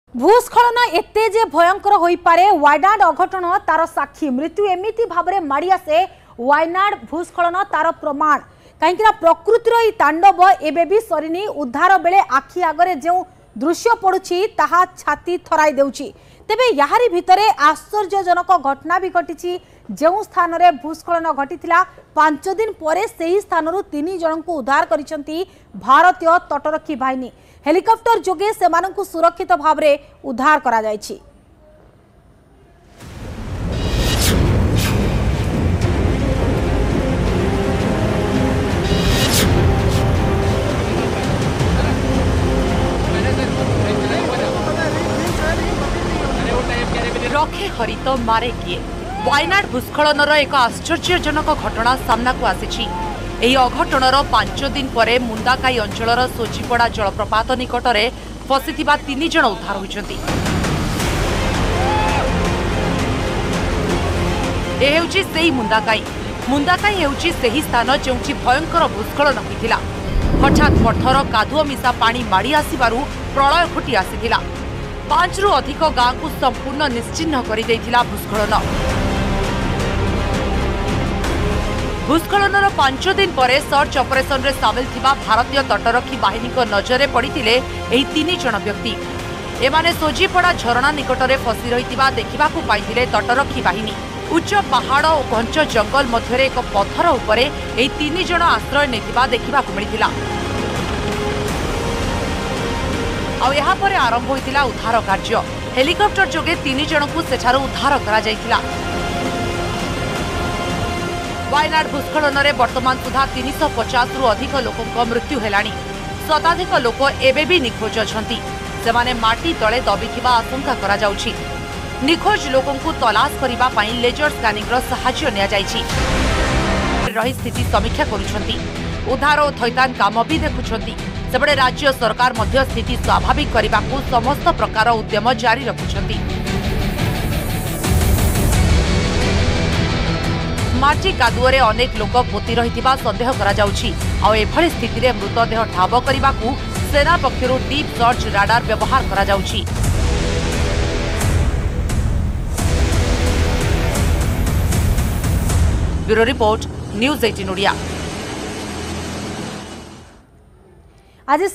କେରଳରୁ ଆସିଲା ଛାତିଥରା ଭିଡିଓ | Rescue Operation In Wayanad landslide | Kerala Landslide Video
Search and rescue operations ongoing in the landslide-affected areas in Kerala's Wayanad. Drone visuals of Landslide hit Chooralmala area of Wayanad..Landslides triggered by heavy rainfall sent flows of mud and debris rushing down hills, burying villages and settlements. The tragedy is the worst disaster in Kerala after the 2018 floods, with experts attributing the landslides to weeks of heavy rains that softened the soil and eventually led to devastating landslides.